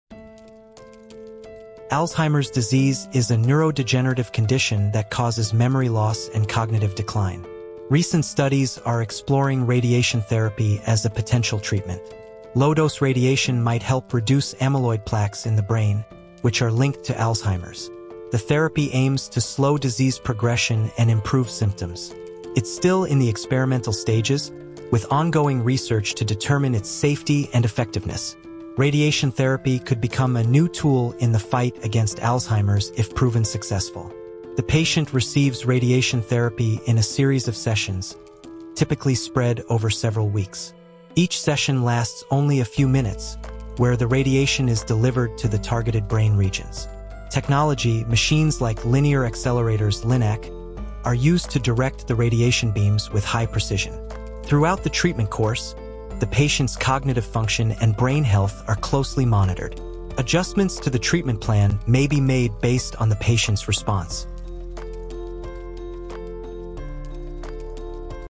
Alzheimer's Disease Treatment - Radiation Therapy
Alzheimer's Disease is a progressive neurodegenerative disorder that primarily affects memory, thinking, and behavior. It is the most common cause of dementia, usually occurring in older adults. Alzheimer's involves the accumulation of amyloid plaques and tau tangles in the brain, which disrupt communication between neurons and lead to cell death.

Symptoms:
Memory loss (especially recent events)
Confusion with time or place
Difficulty in problem-solving or completing familiar tasks
Changes in mood or personality
Trouble with language or communication
Causes:
The exact cause of Alzheimer’s is unknown, but risk factors include:

Age (main risk factor)
Genetics (family history or certain genes like APOE-e4)
Lifestyle factors (poor diet, lack of physical exercise)
Brain injury and heart health issues.

Radiation Therapy is not a standard treatment for Alzheimer’s disease (AD), but some emerging research explores its potential role. Alzheimer’s is primarily treated with medications and lifestyle interventions aimed at managing symptoms and slowing disease progression. However, researchers are investigating whether low-dose radiation therapy (LDRT) could be beneficial in treating Alzheimer’s.